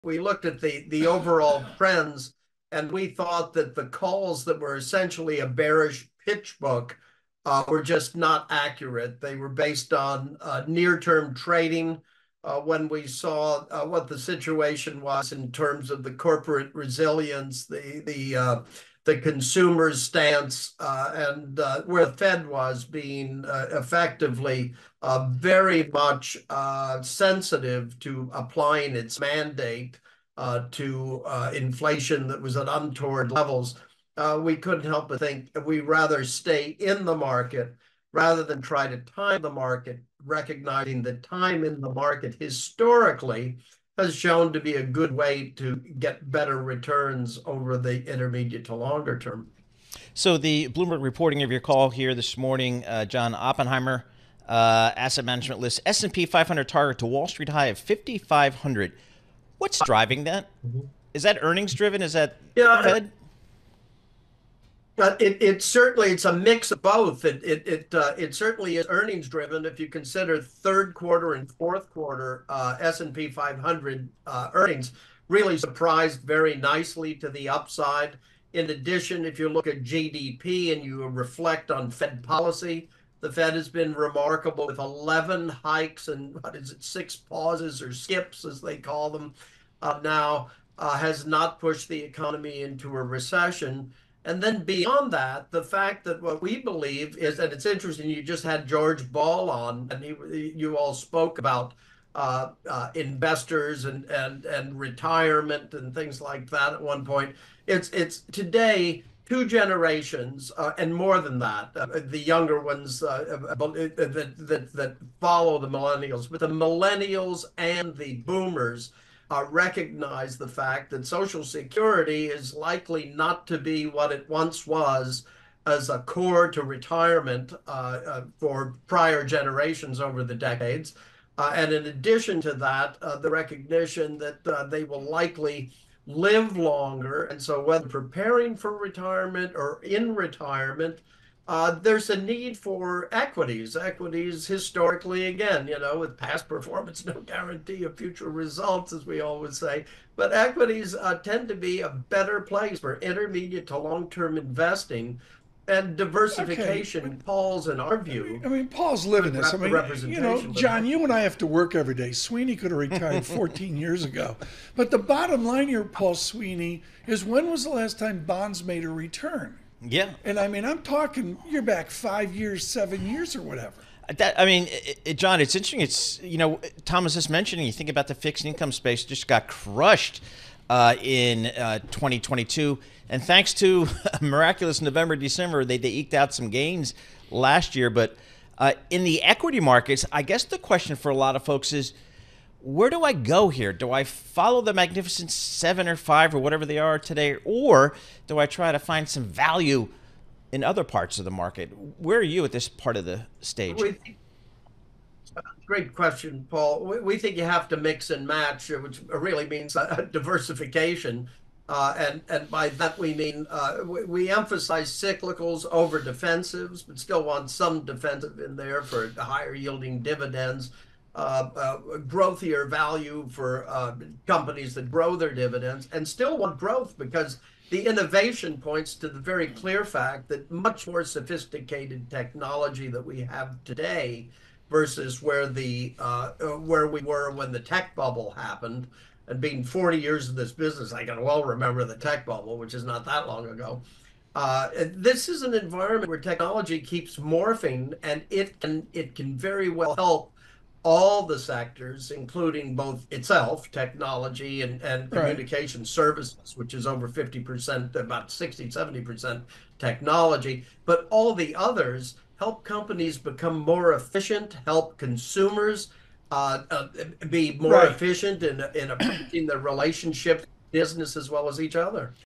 Why Oppenheimer Raised Its S&P 500 Target to 5,500
Oppenheimer Chief Investment Strategist John Stoltzfus details his firm's decision to raise its S&P 500 forecast and explains why he believes younger investors should be in the market for the long term instead of trying to time it. John speaks with Tom Keene and Paul Sweeney on Bloomberg Radio.